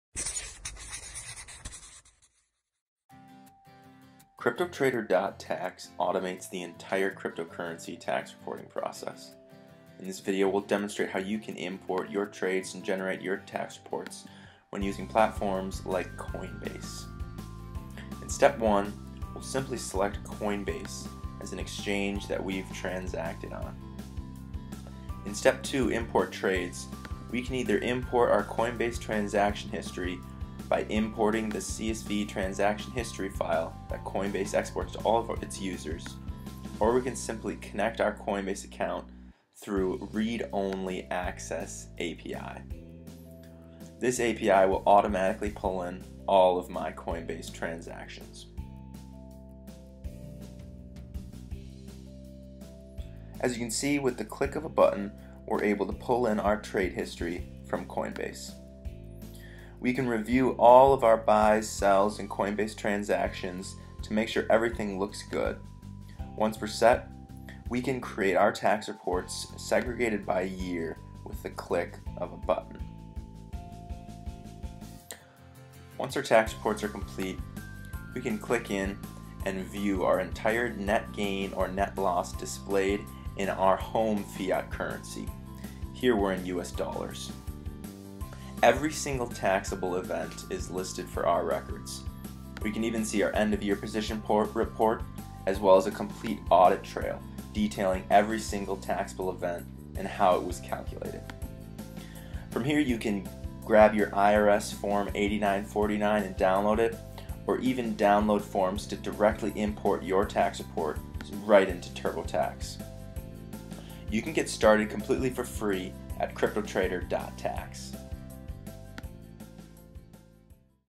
Coinbase Tax Reporting - CryptoTrader Tax
Automate your cryptocurrency tax reporting with CryptoTrader.Tax. This video demonstrates how you can use the platform with Coinbase.